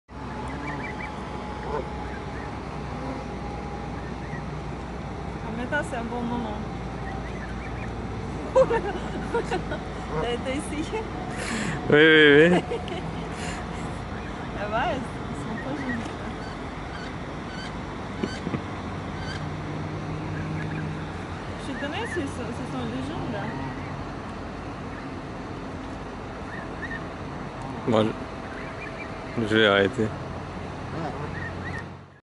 Canada Geese Asking for Food | Stanley Park, Vancouver, BC
These Canada geese gathered around me. They were asking for food. Unfortunately, I didn't bring any treats with me... So, one goose got impatient and started biting me.